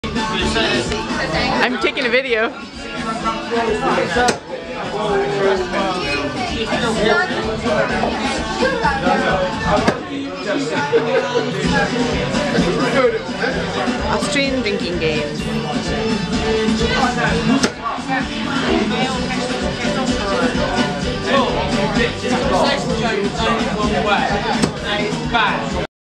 Austrian drinking games - the nail game
A typical Austrian drinking game that we played in the pubs after skiing. The idea is to try to be the first person to get the nail into the log with the opposite end of the hammer but if you lose, you've got to buy everyone a round!